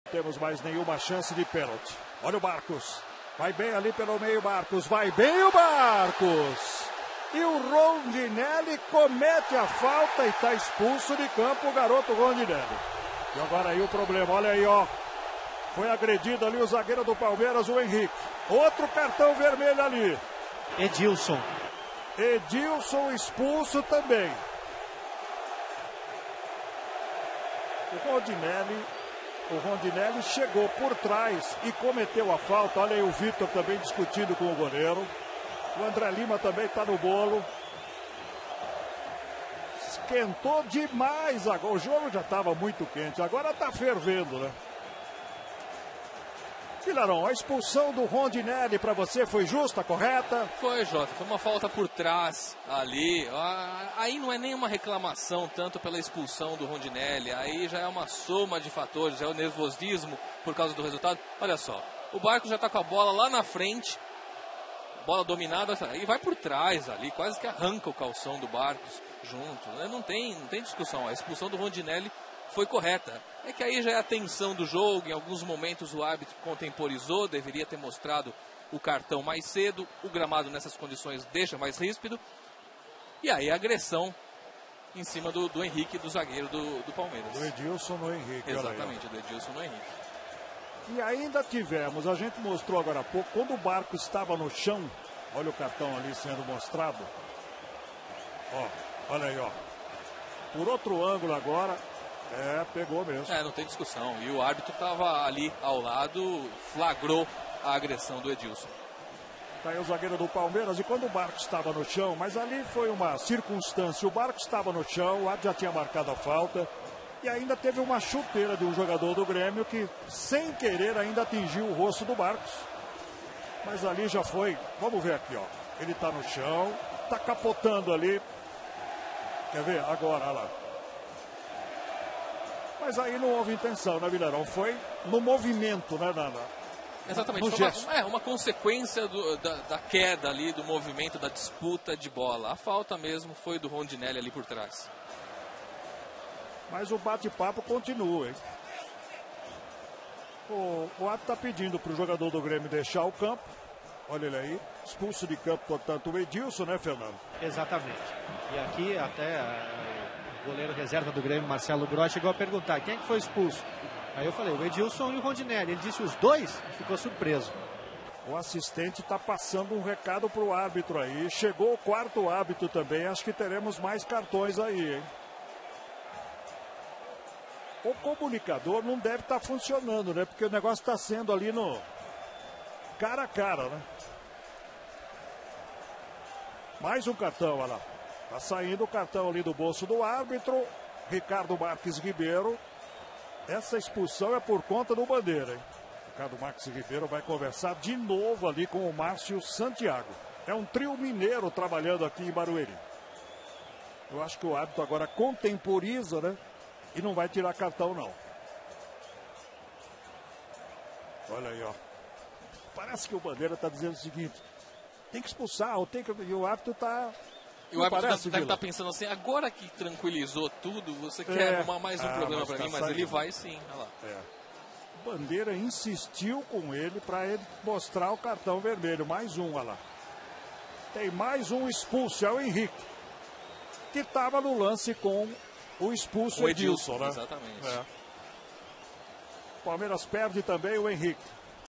Confusão no jogo entre Palmeiras e Grêmio - Copa do Brasil 2012
3 expulsões: Edilson e Rondinelly (GRÊMIO) e Henrique.(PALMEIRAS)